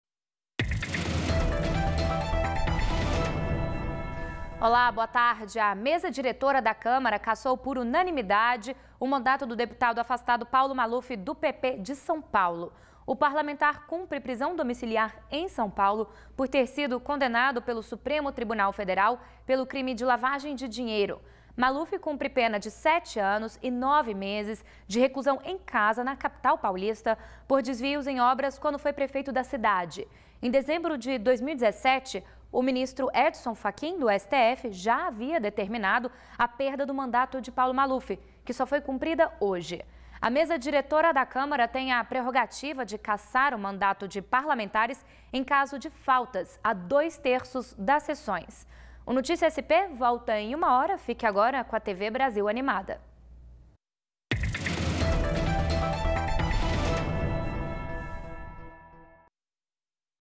Câmara dos Deputados cassa mandato de Paulo Maluf
A Câmara dos Deputados cassou nesta quarta-feira (22) o mandato do deputado federal Paulo Maluf (PP-SP), de 86 anos. A decisão da Mesa Diretora foi unânime.